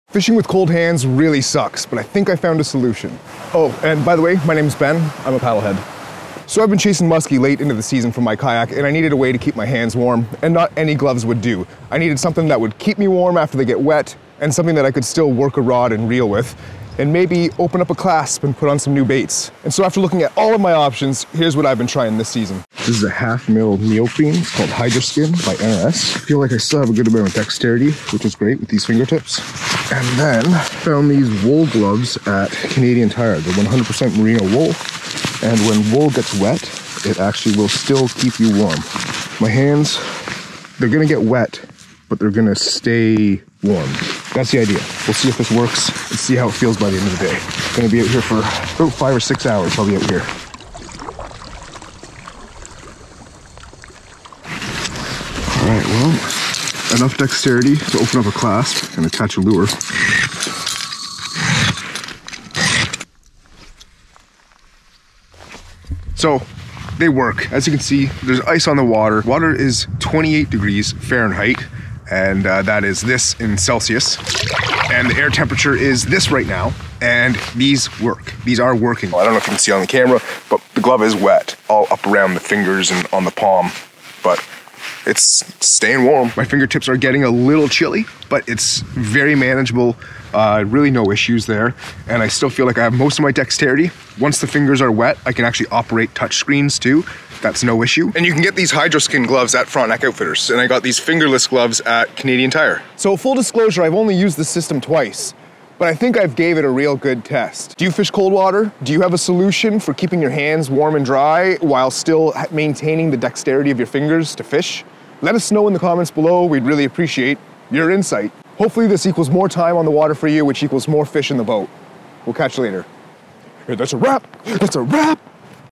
Testing cold weather gloves while kayak fishing for musky
Ben tries out a combination of gloves for cold-weather fishing while targeting muskies in the fall.

Check out these links for some of the gear we use and companies we support.

🐠 Soft Plastic Fishing Baits made in Ontario!